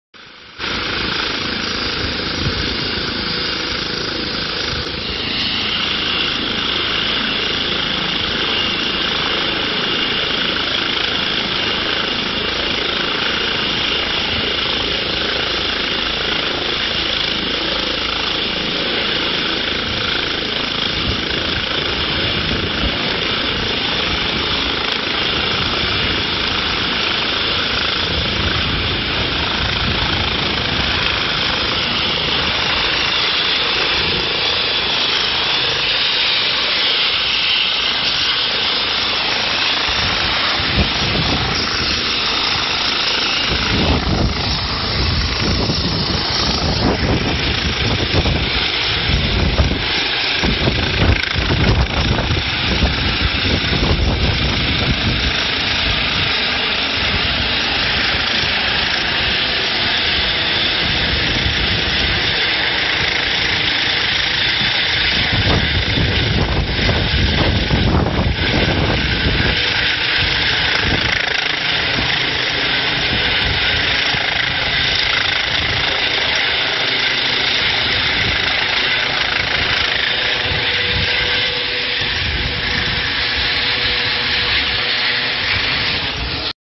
OSP Milicz-Zabezpieczanie lądowania śmigłowca
OSP Milicz zabezpiecza teren podczas lądowania śmigłowca  LPR RATOWNIKA 13 na miejsce wypadku drogowego po osobę poszkodowaną w dniu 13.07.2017 rok